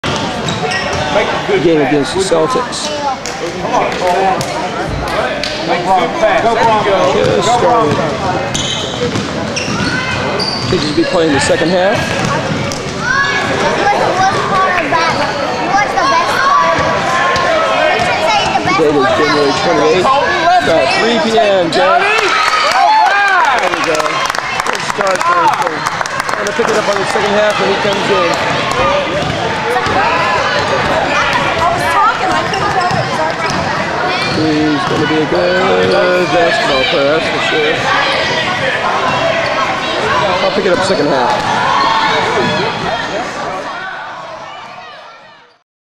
J.J. rides the bench (Sort of...) during a Basketball Game.
In Upward, and in the vein of fairness, every child gets a chance to play the game at hand, no matter what their skill level. This is a good example of those of you with children on the bench...